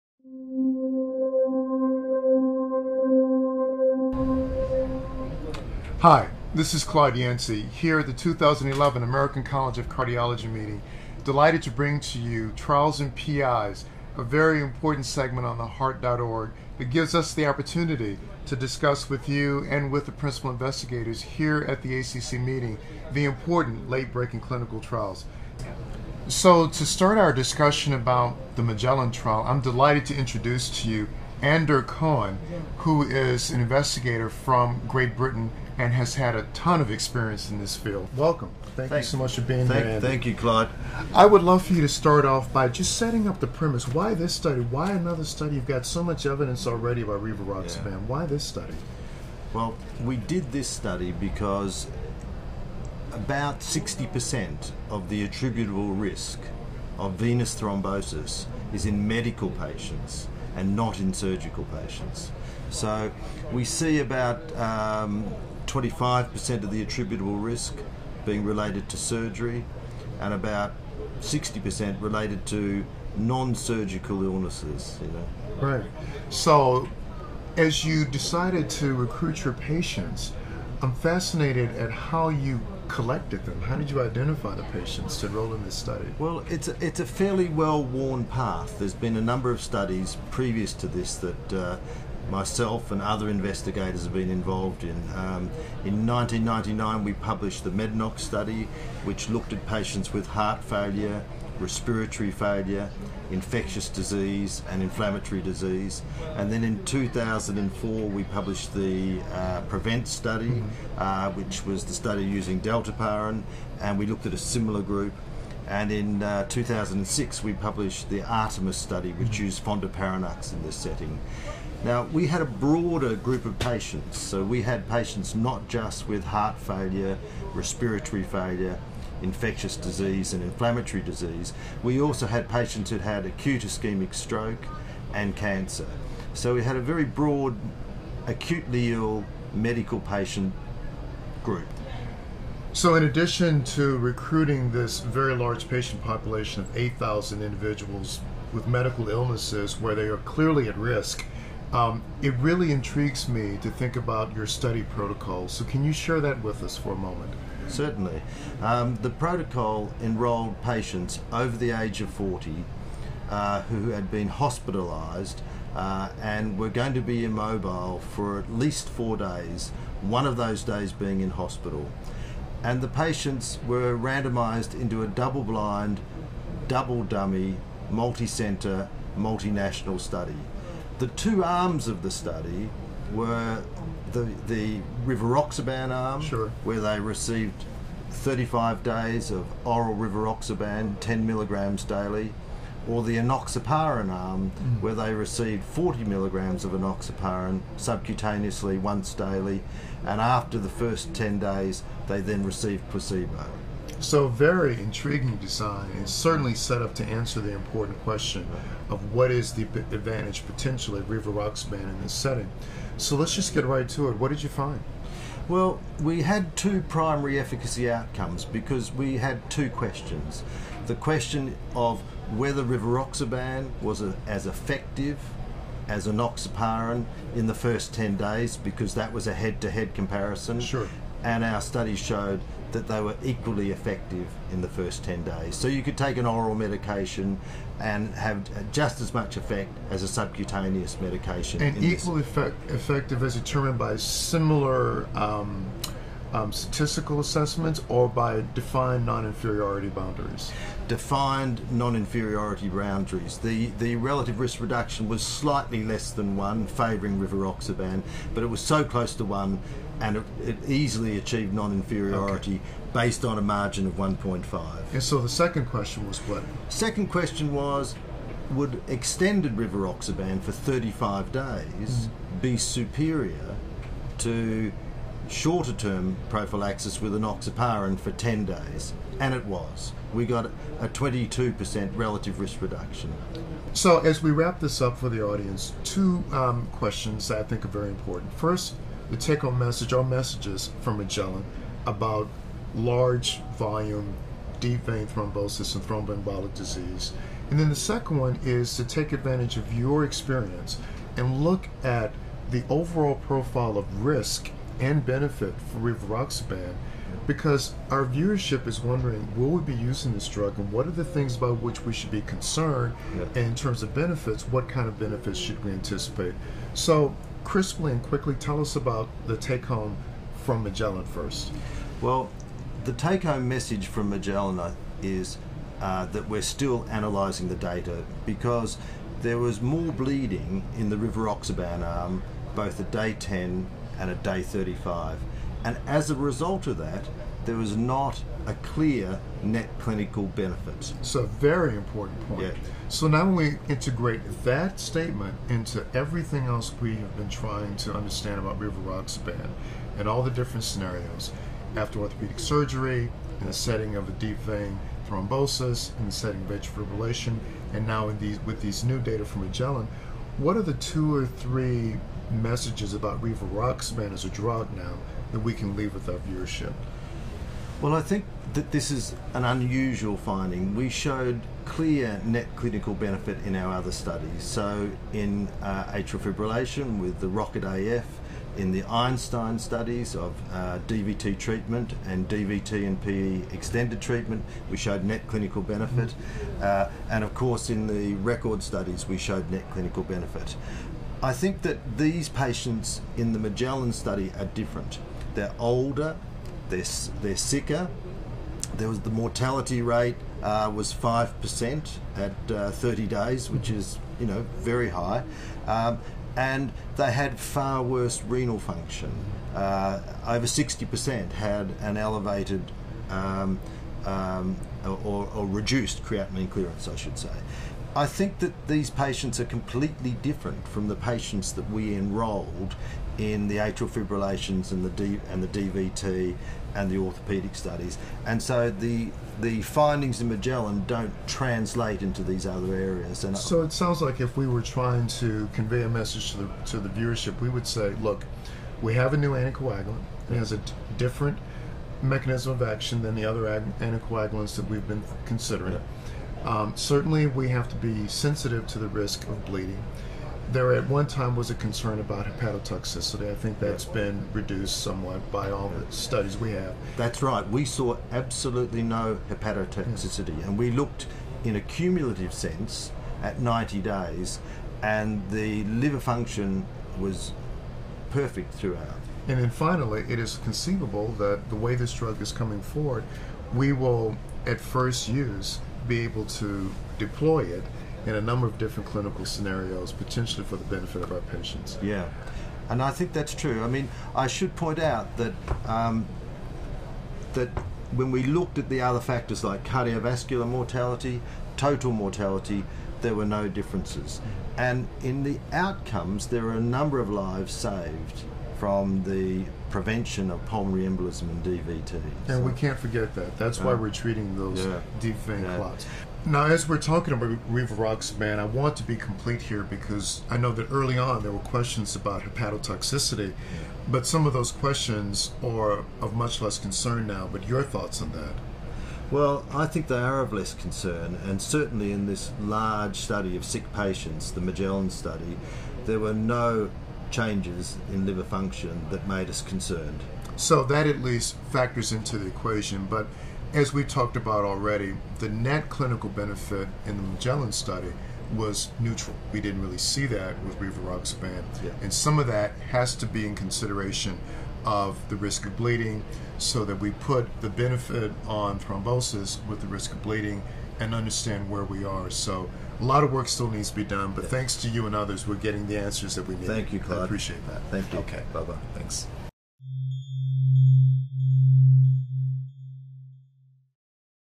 Rivaroxaban vs enoxaparin for the Prevention of Venous Thromboembolism..
Dr Alexander Cohen (King's College, London, UK) discusses rivaroxaban compared with enoxaparin for the prevention of venous thromboembolism in acutely ill medical patients.

Filmed during ACC 2011 in New Orleans, LA on April 4.